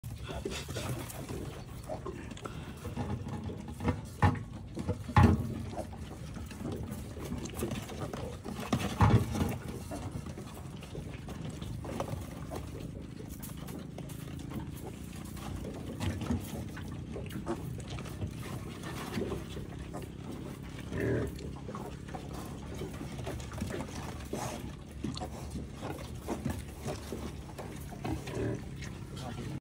So cute little pigs New born baby pig so cute Sleeping with their mother 15
This Videos is Showing the Pig surving in the farm
The Farm Raising Pig on top of water
Beautiful farming of Pig
Modern Farming Pig Farm